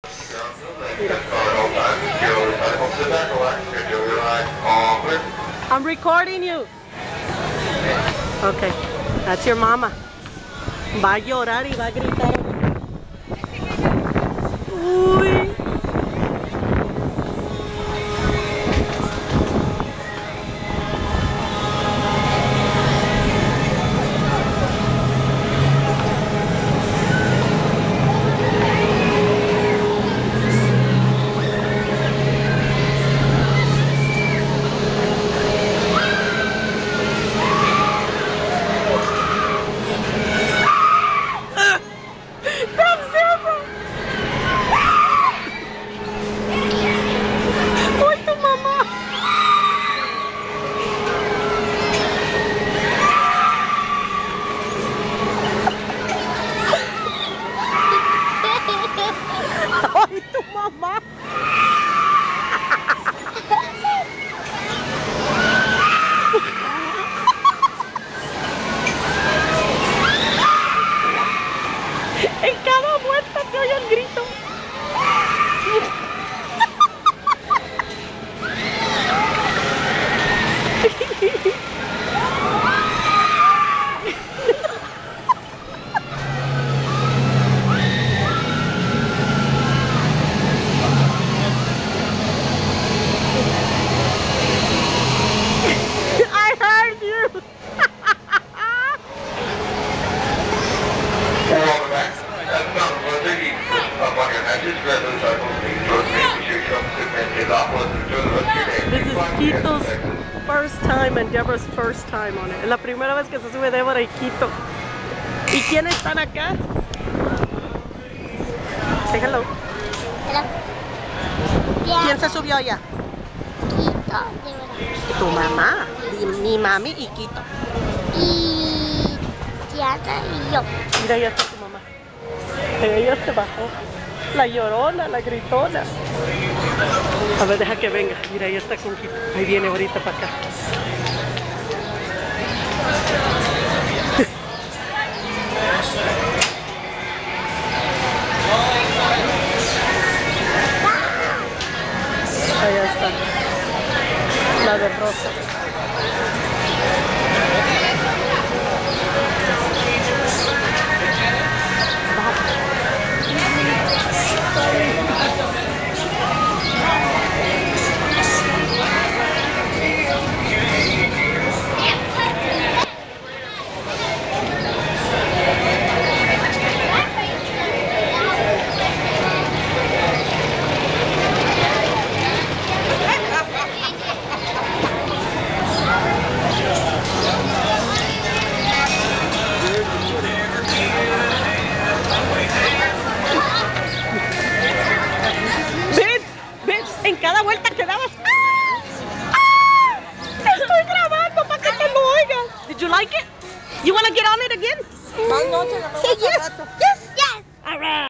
Six Flags Fiesta TX Debra And baby Hector
My sister and her son riding the Wagon Wheel and she is screaming her head off, very funny.